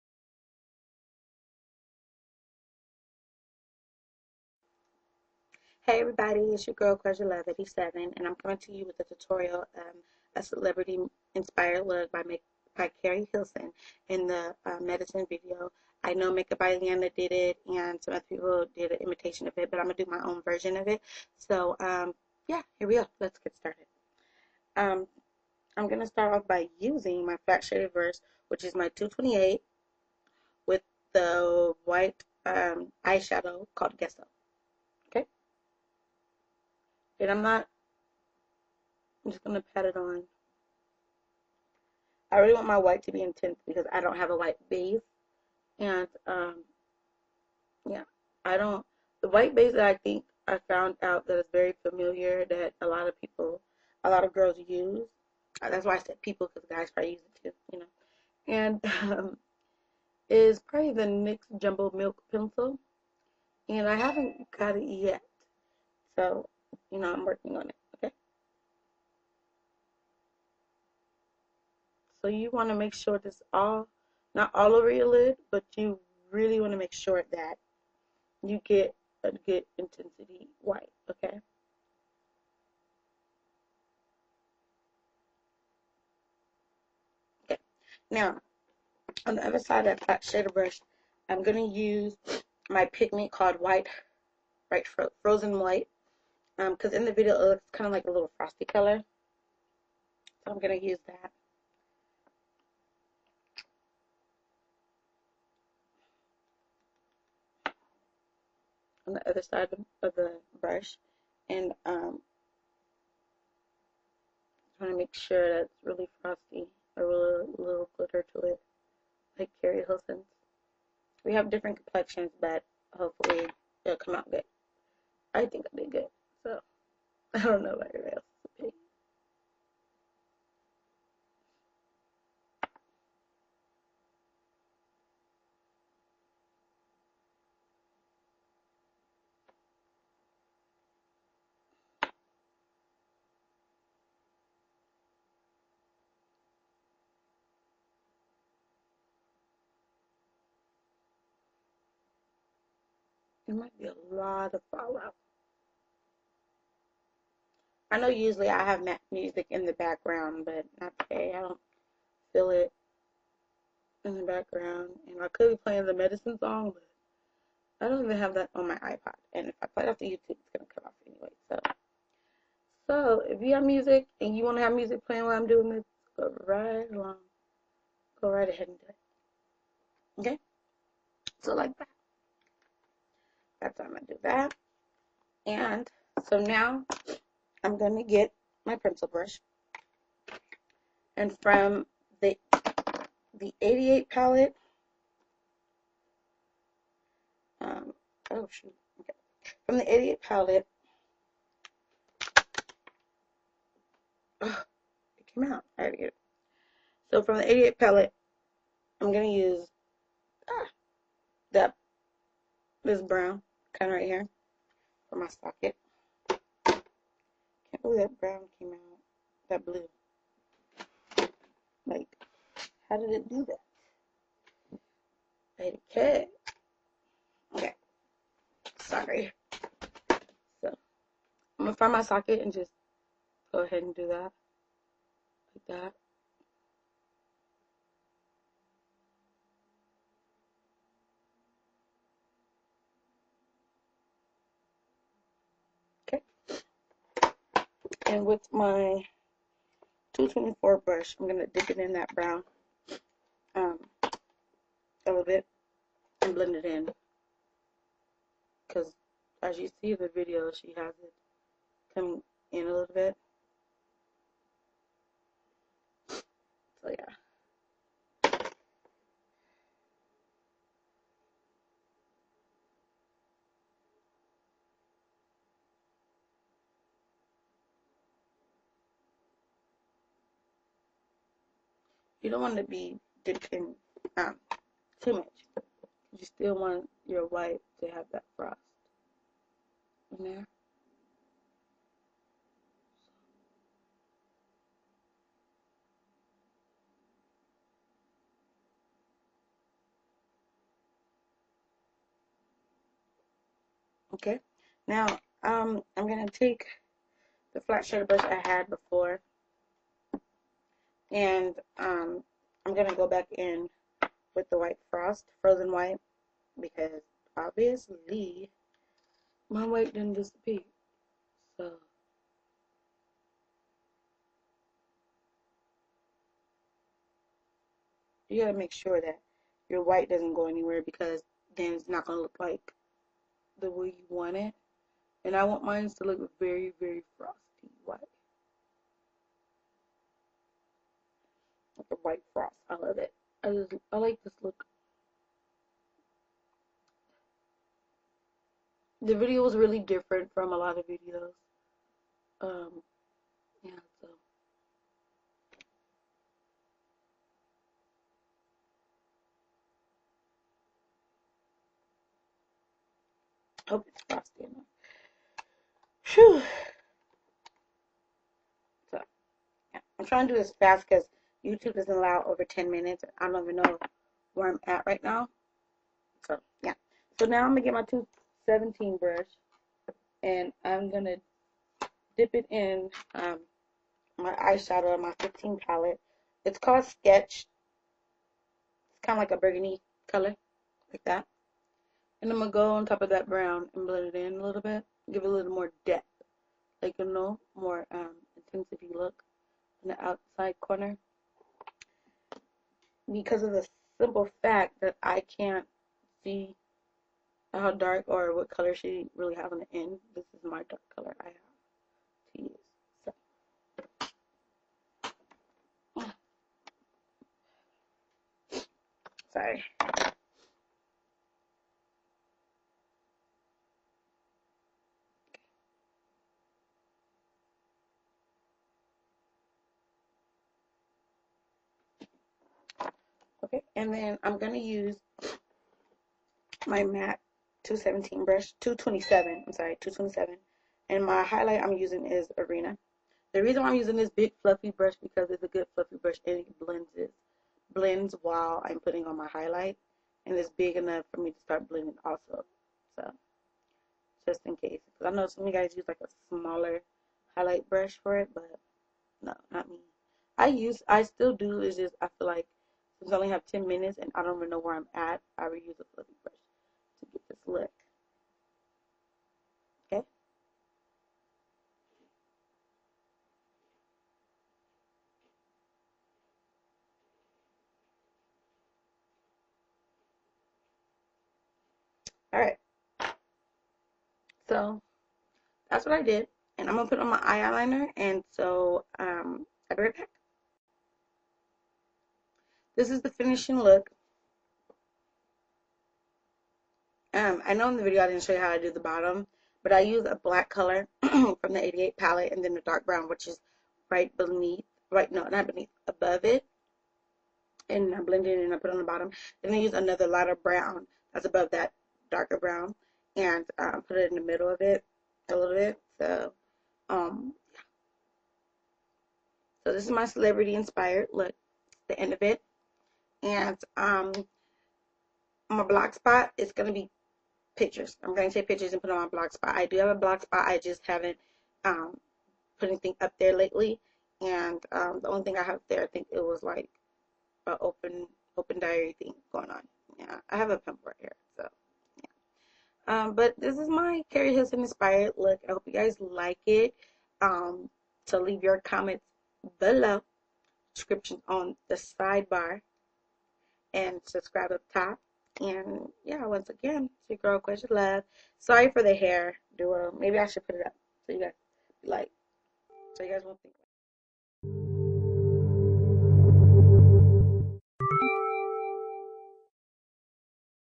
celeb inspried look keri baby
IM DOING THE MAKE UP BY KERI HILSON FROM THE PLIES VIDEO I HOPE YOU ENJOY.

TWITTER.COM/QUASHA_B

FACEBOOK.COMQUASHAB

FACE/
LOOSE POWDER NW35
SKIN FINISH MEDIUM DEEP

EYES/
PAINT POT-GROUNDWORKS
88 PALLETTE
SKETCH
HIGHLIGHT-ARENA
EYELINER-H.I.P 905
MASCARA-ZOOM FAST BLACK LASH

CHEEKS/
FLUER POWER

LIPS/
LINER-SOAR
LIPSTICK-UP THE AMP
LIPGLASS-ELECTRIC EDGE